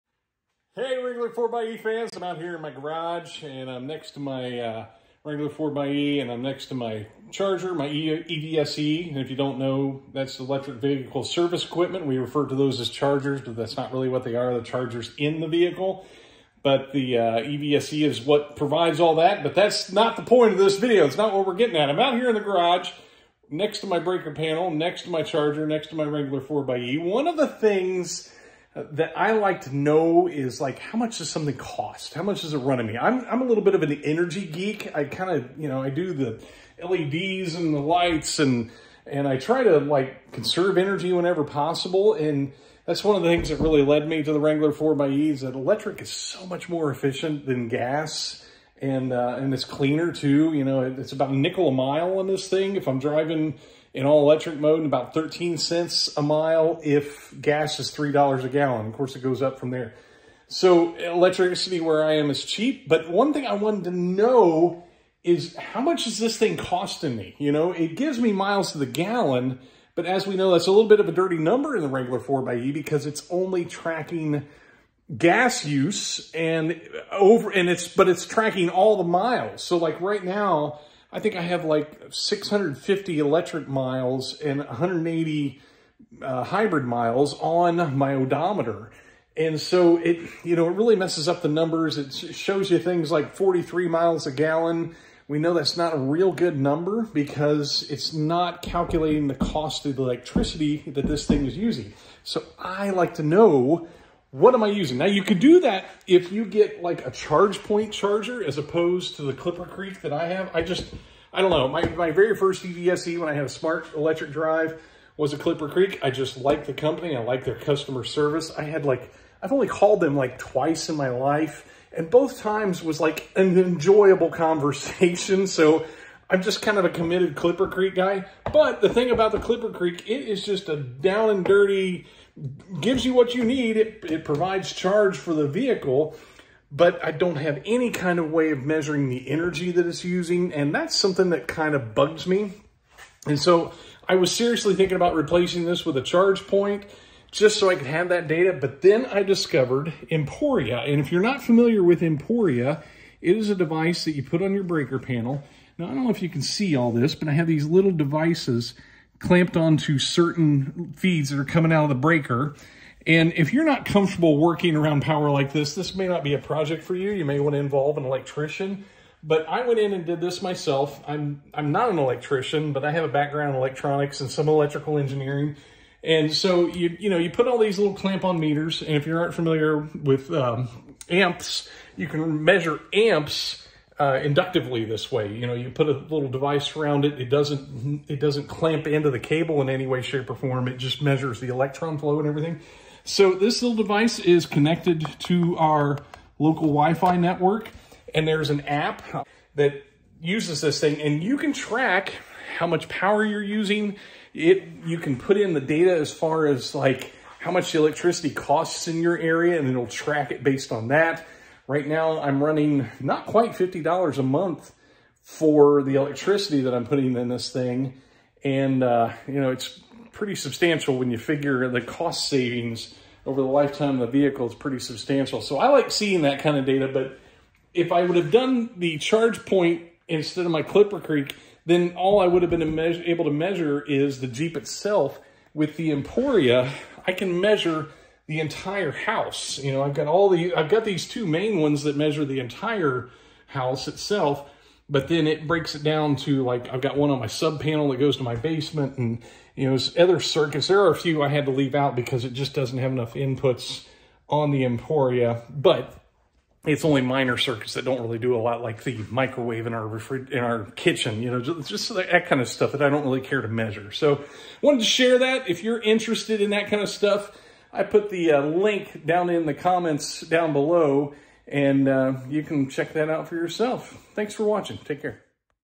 How to measure your electric use in the Wrangler 4XE
One of the things I wanted to be able to do in the Wrangler 4XE was measure my electrical energy usage. I picked up the Emporia. So not only can I see how much it costs me to charge the Wrangler 4XE, I can also see what our usage in the rest of the house looks like too.

Here is the link to the Emporia on Amazon